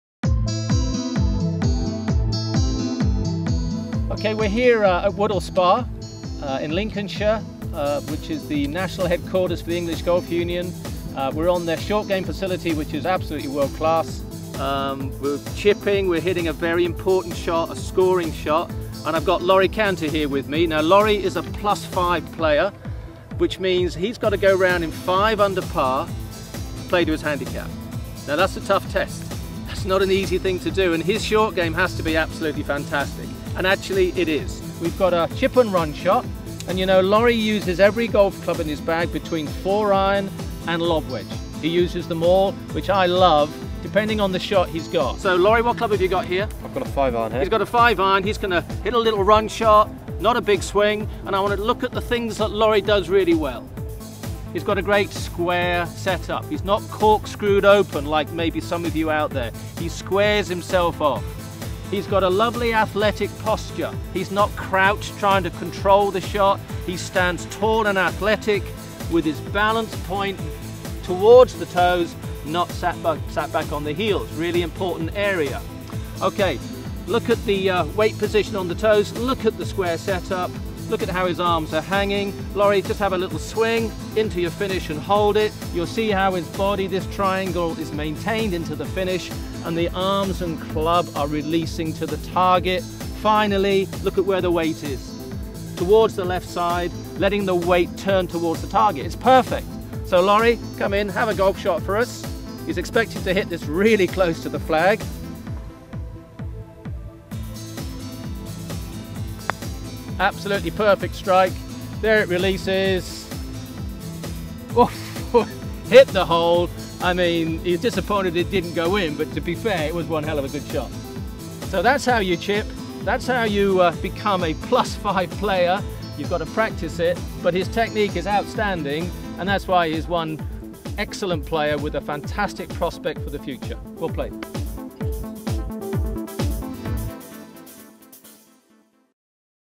Chipping tips with Gary Smith (2011)
Gary Smith offers some tips on chipping to help golfers achieve better scores.